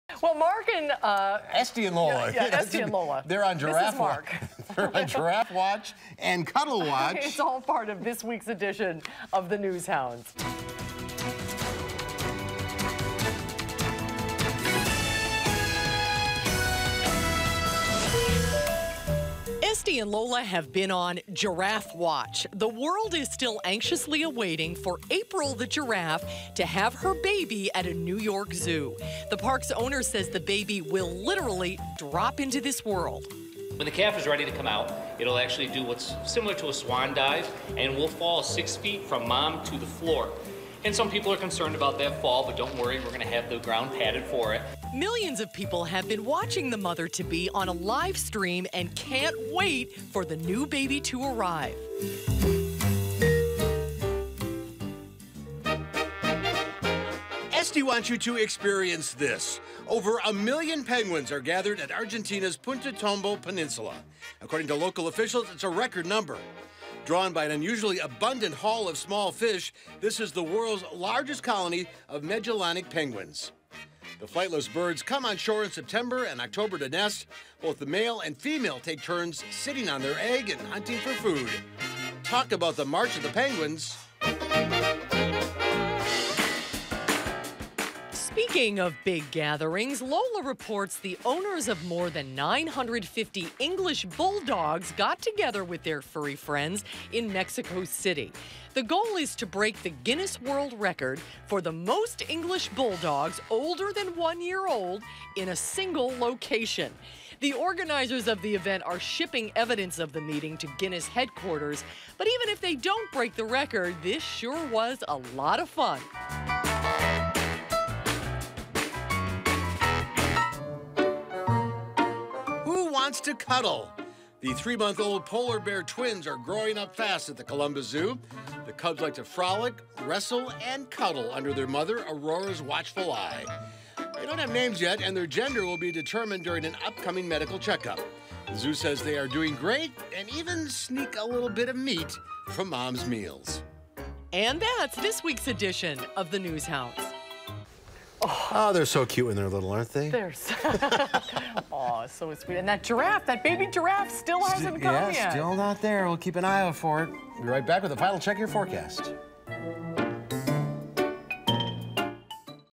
News Hounds: Giraffe watch and Cuddle watch
Estie and Lola bring us the latest in animal news.